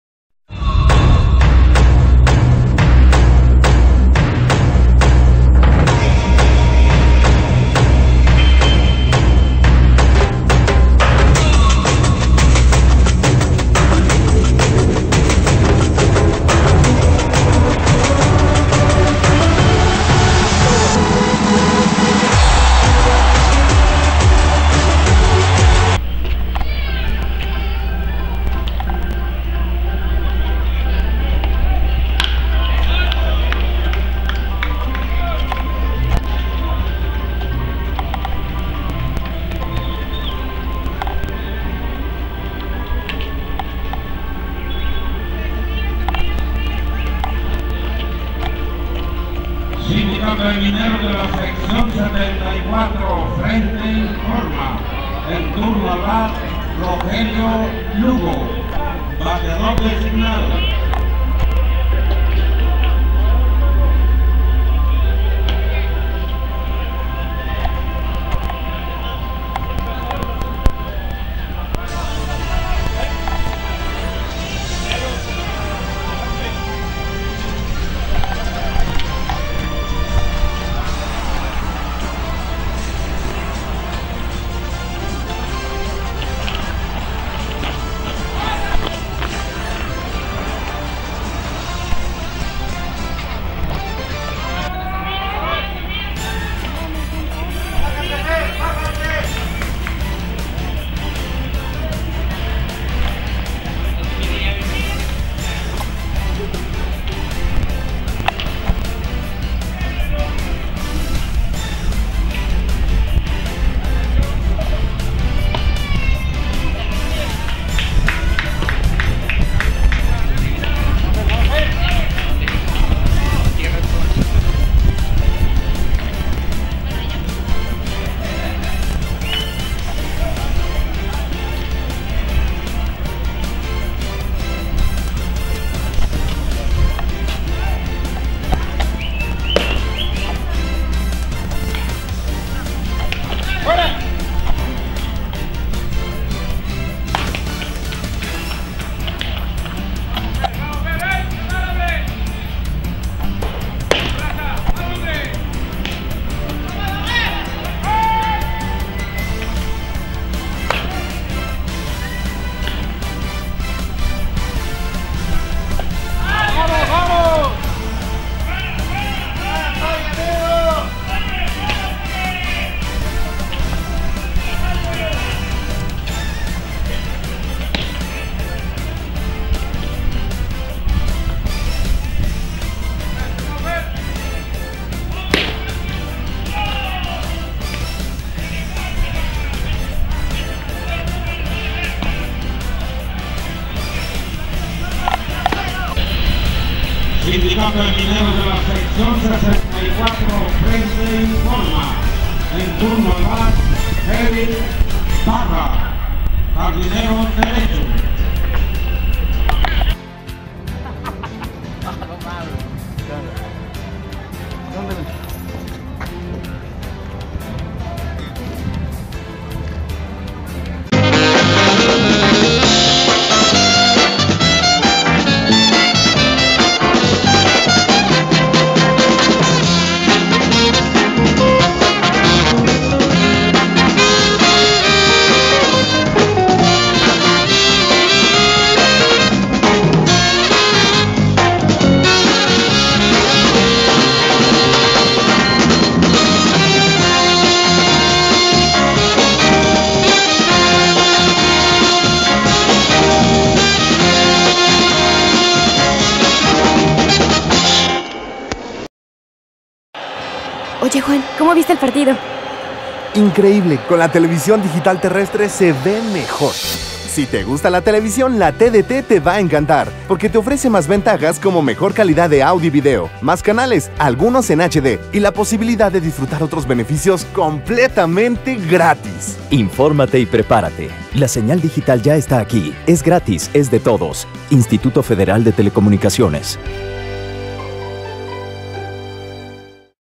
LOSFANATICOS 119
ASPECTOS DEL JUEGO REALIZADO ENTRE LOS VAQUEROS DE MADERO Y LOS MINEROS SECCION 74 EFECTUADO EN EL PARQUE DE LA SECCION 74 EN PEÑOLES, TORREON.